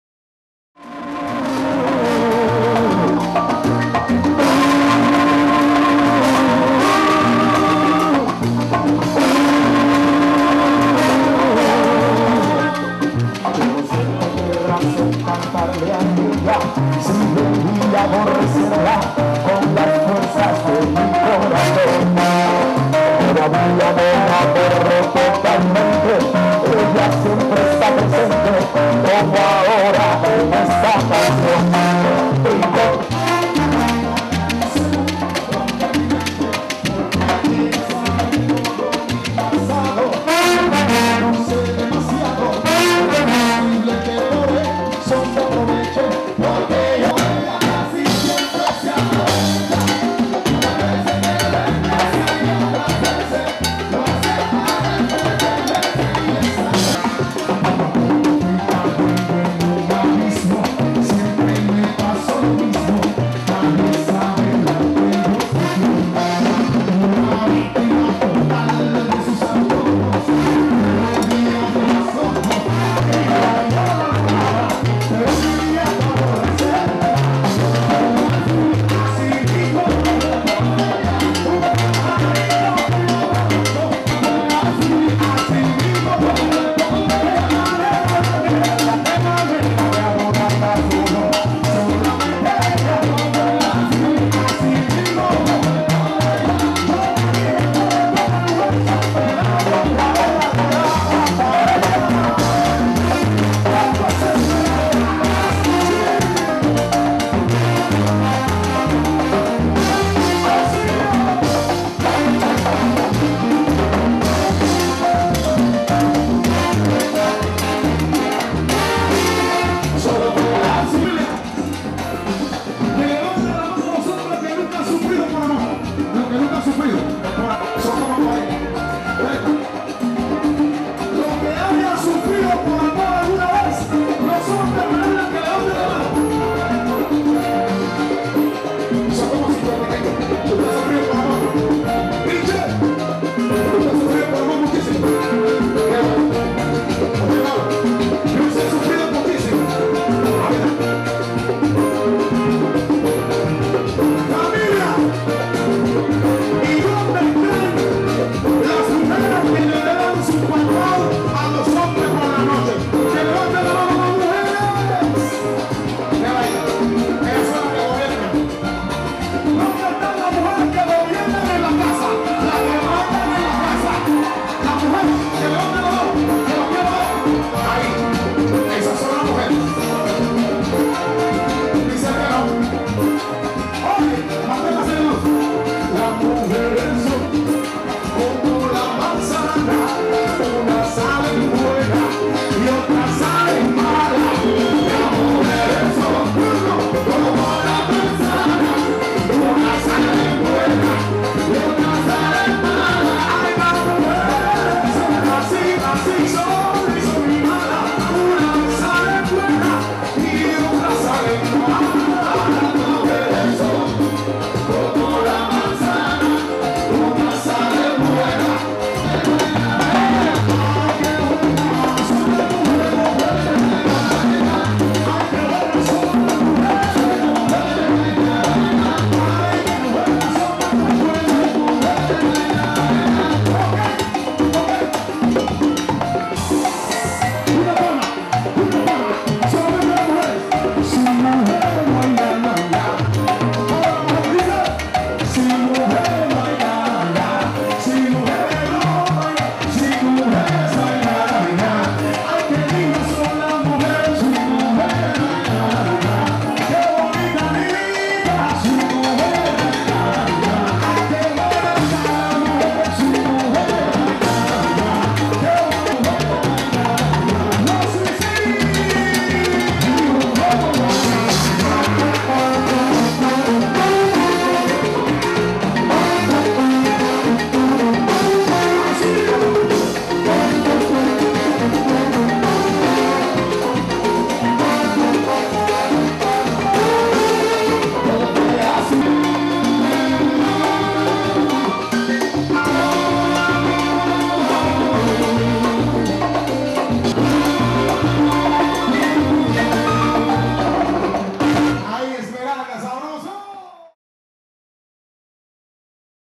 LOS KARACHIS EN ESMERALDAS
Concierto de la Orquesta Internacional Karachi en los carnavales de la ciudad de Esmeraldas en Febrero del 2009